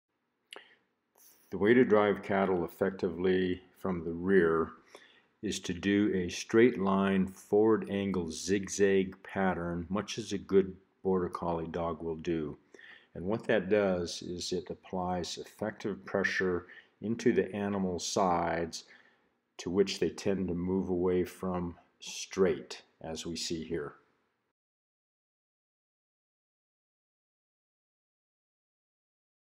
Driving cattle from the rear
Low-stress livestock handling expert Whit Hibbard describes good gathering and driving techniques. Read more in the article that goes with this video It published on January 28, 2020 and will be visible then.